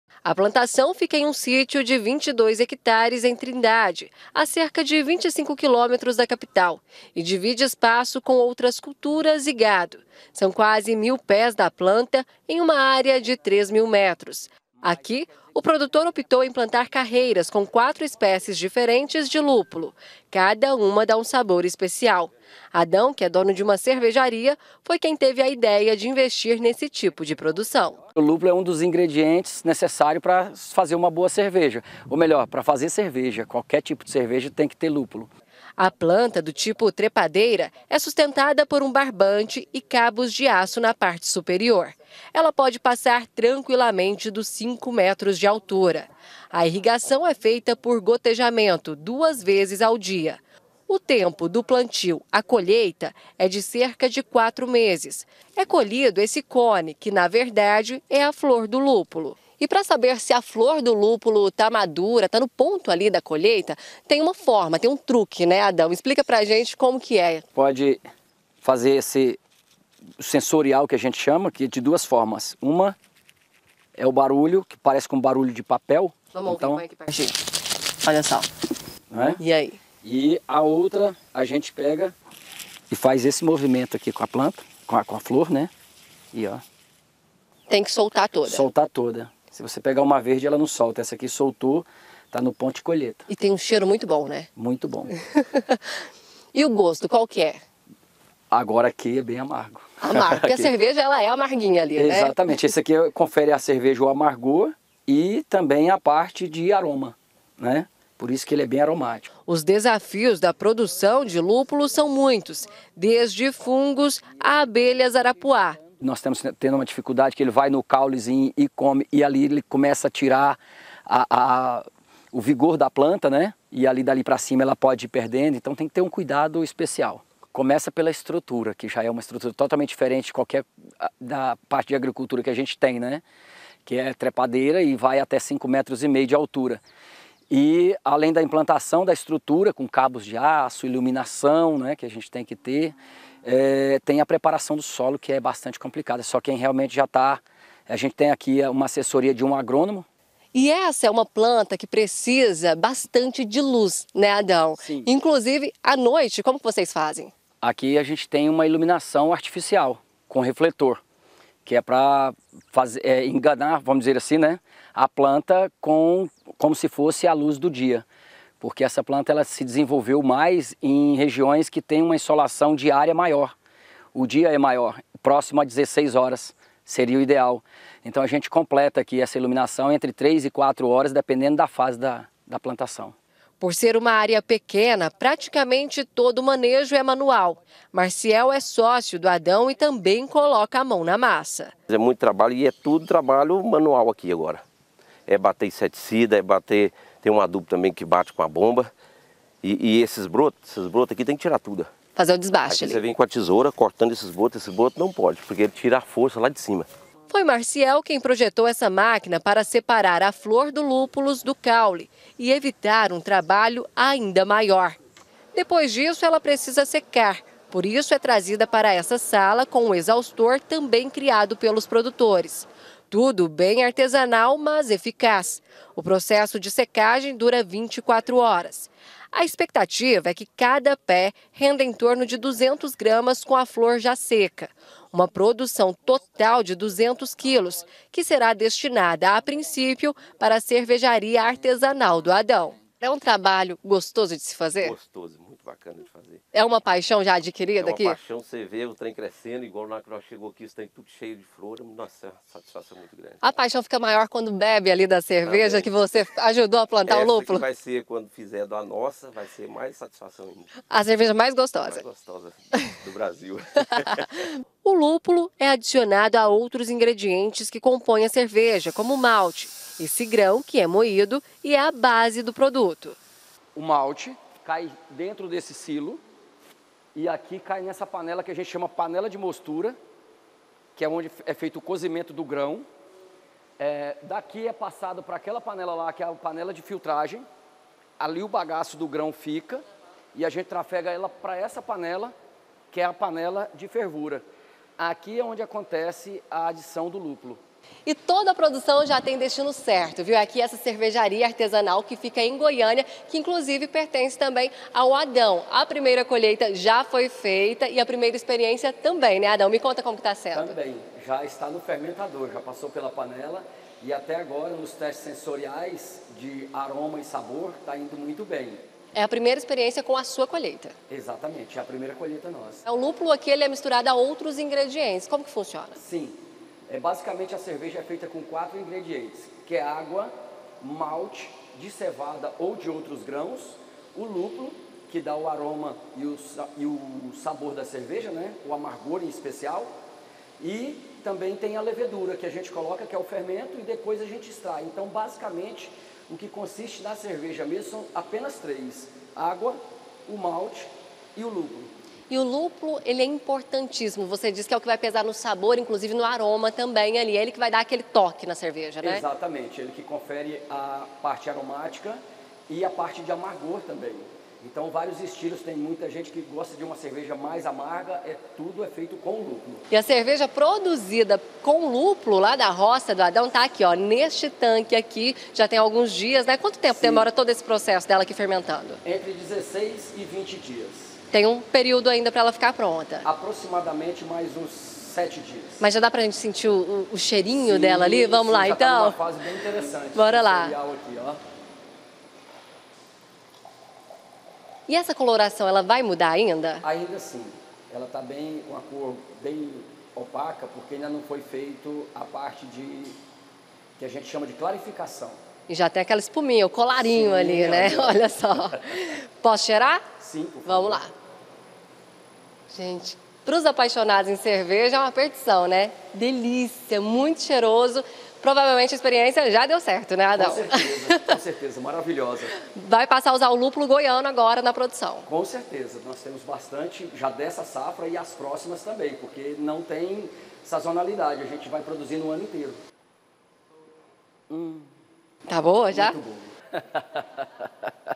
DE PLANTAÇÃO À CERVEJARIA: LÚPULO É O QUE GARANTE A QUALIDADE DA CERVEJA
VOCÊ JÁ VIU UMA PLANTAÇÃO DE LÚPULO, AQUELE INGREDIENTE USADO PRA FAZER CERVEJA? AQUI EM GOIÁS, A PRODUÇÃO NÃO É COMUM. MAS NÓS DESCOBRIMOS UM SÍTIO EM TRINDADE QUE APOSTOU NO PLANTIO E ESTÁ DANDO MUITO CERTO.